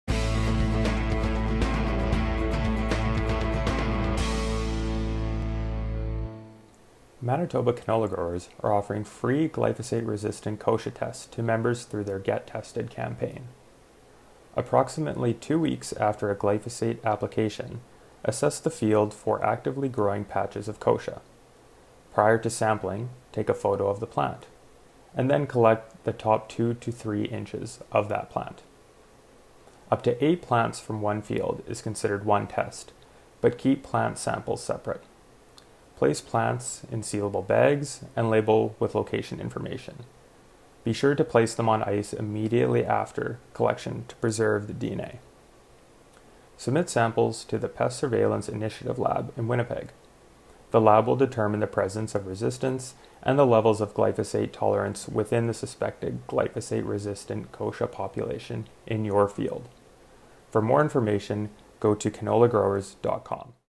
Glyphosate Resistant Kochia Testing
Learn more about how MCGA Members can claim free glyphosate resistant kochia testing through the GetTested campaign and the Pest Surveillance Initiative.